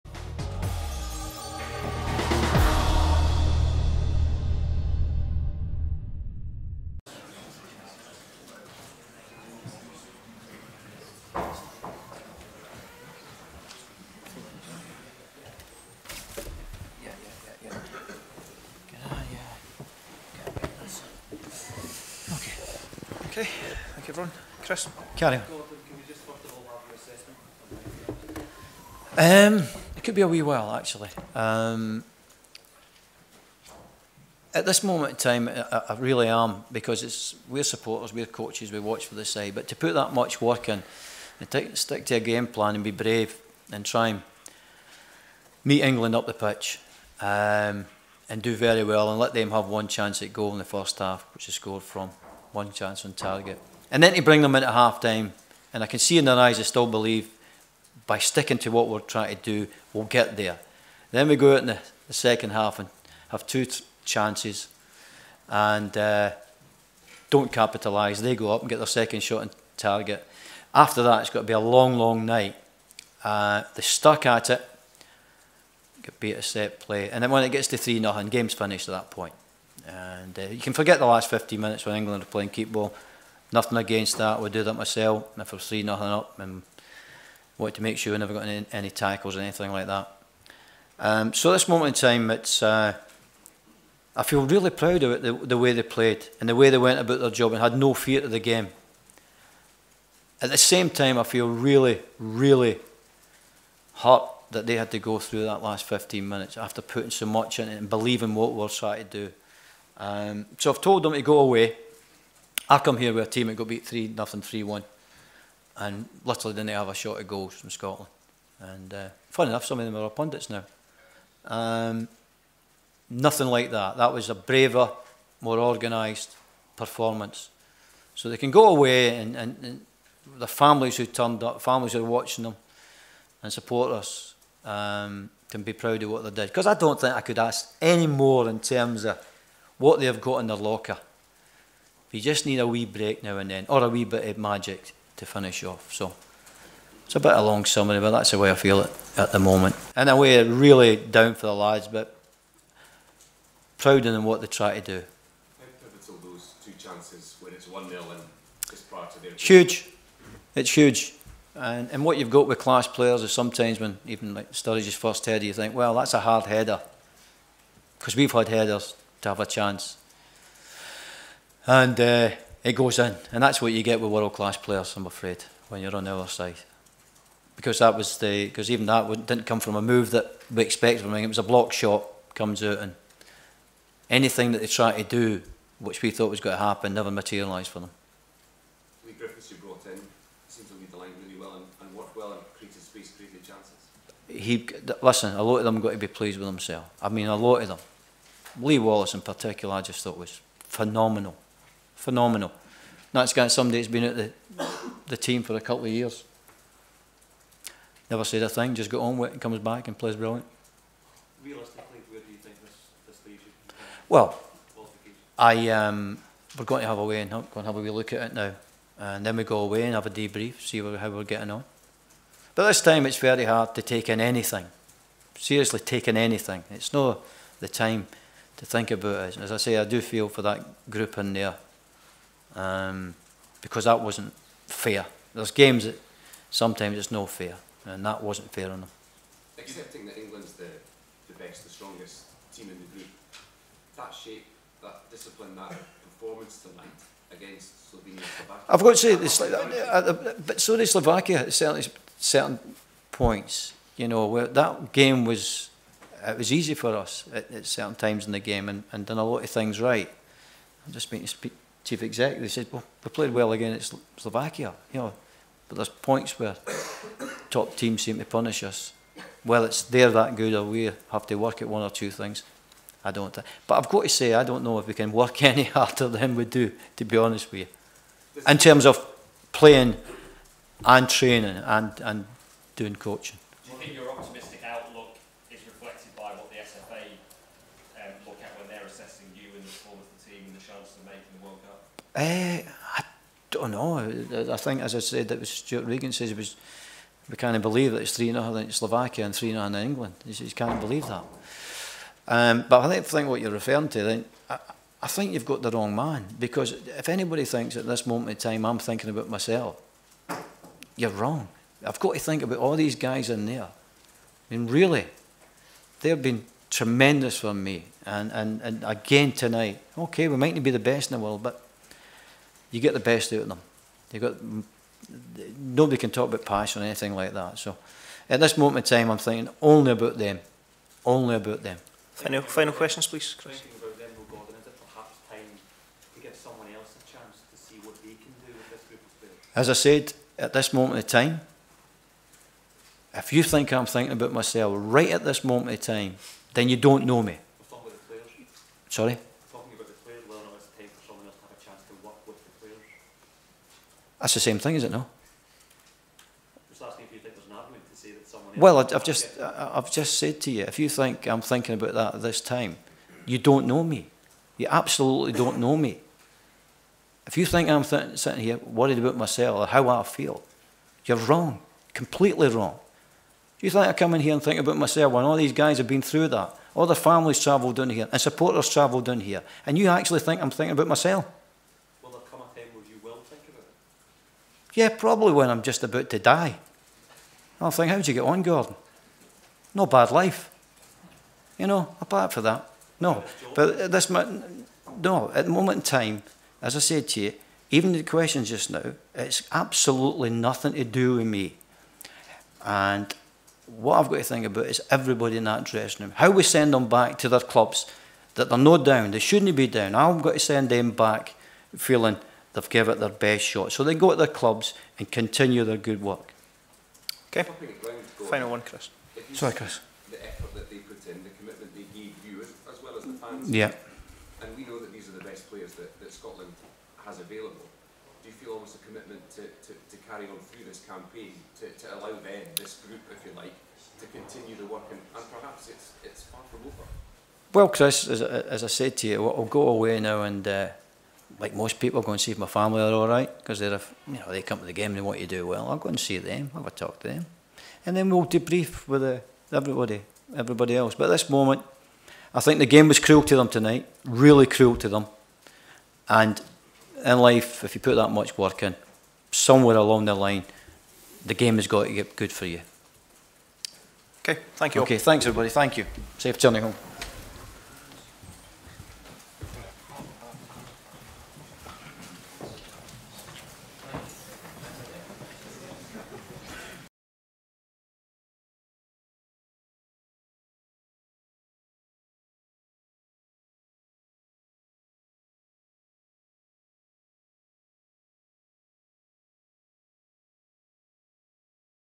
England 3-0 Scotland - Gordon Strachan Full Post Match Press Conference
Scotland manager Gordon Strachan speaks to the press following his side's 3-0 defeat to England in their World Cup qualifying match at Wembley.

Thanks